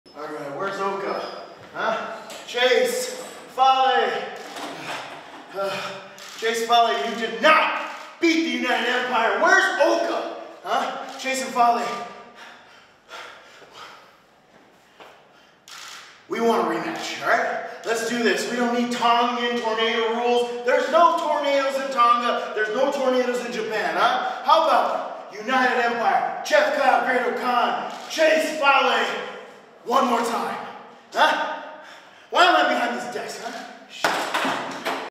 Jeff Cobb | WRESTLING DONTAKU 2022, 5/1/22
May 1, 2022 “WRESTLING DONTAKU 2022”
Fukuoka・Fukuoka Dome
5th match: IWGP TAG TEAM CHAMPIONSHIPS
YOSHI-HASHI and Hirooki Goto 🆚 Jeff Cobb and Great-O-Khan 🆚 Bad Luck Fale and Chase Owens